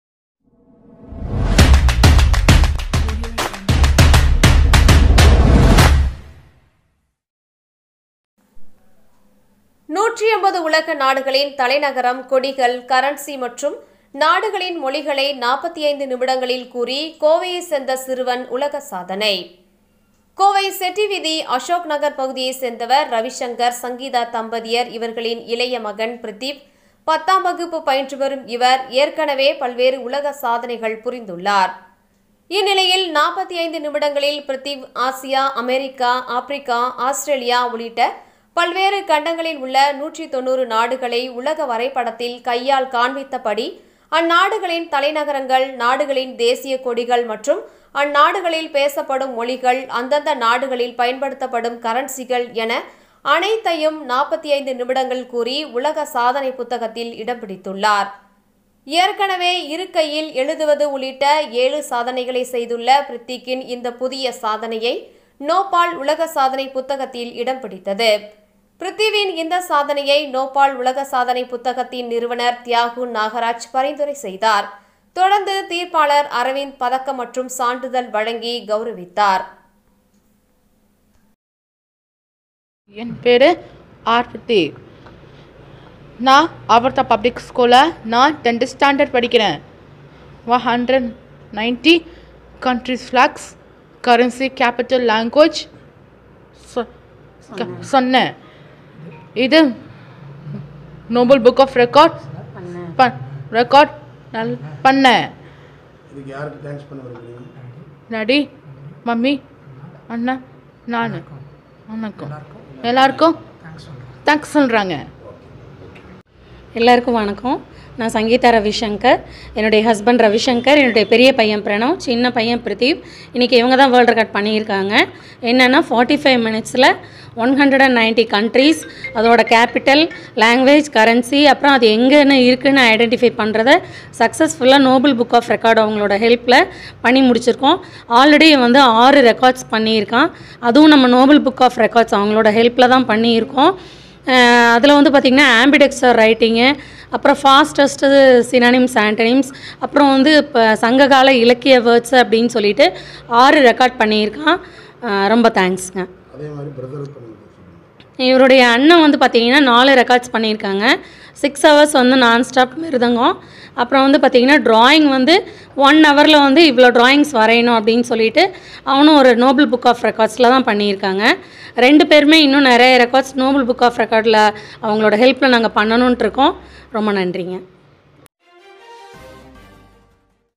180 capital cities of the world, flags, currency, languages countries 45 minutes is an achievement
கோவை, செட்டி வீதி அசோக் நகர் பகுதியைச் சேர்ந்தவர் ரவிசங்கர், சங்கீதா தம்பதியர். இவர்களின் ,இளைய மகன் பிரித்திவ்.கல்லூரி பத்தாம் வகுப்பு பயின்று வரும் இவர், ஏற்கனவே  பல்வேறு உலக சாதனைகள் புரிந்துள்ளார். இந்நிலையில்  45 நிமிடங்களில்  .பிரித்திவ், ஆசியா அமெரிக்கா ஆப்பிரிக்கா ஆஸ்திரேலியா உள்ளிட்ட பல்வேறு கண்டங்களில் உள்ள 190 நாடுகளை உலக வரைபடத்தில் கைகளால் காண்பித்தபடி அந்நாடுகளின் தலைநகரங்கள்,நாடுகளின் தேசிய கொடிகள் மற்றும் அந்நாடுகளில் பேசப்படும் மொழிகள் அந்தந்த நாடுகளில் பயன்படுத்தப்படும் கரன்சிகள் என அனைத்தையும் 45 நிமிடங்களில் கூறி உலக சாதனை புத்தகத்தில் இடம் பிடித்துள்ளார்.ஏற்கனவே இரு கைகளில் எழுதுவது உள்ளிட்ட ஏழு  சாதனைகளை செய்துள்ள ப்ரித்திக்கின் இந்த புதிய சாதனை ,நோபல் உலக சாதனை புத்தகத்தில் இடம் பிடித்தது... பிரித்திவின் இந்த சாதனையை நோபள் உலக சாதனை புத்தகத்தின் நிறுவனர் தியாகு நாகராஜ் பரிந்துரை செய்தார். தொடர்ந்து  தீர்ப்பாளர் அரவிந்த்  பதக்கம் மற்றும் சான்றிதழ் வழங்கி கவுரவித்தார்.